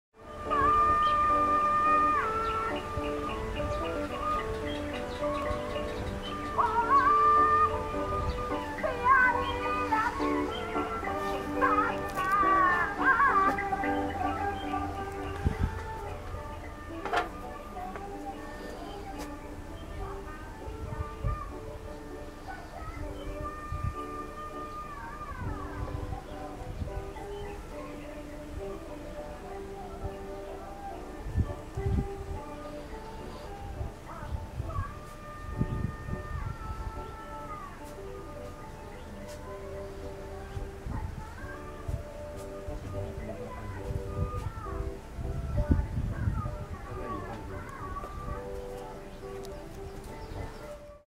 鹿港貝殼廟
魔性的BGM跟十八羅漢，所以我要稱之為『怪春先生的異想世界』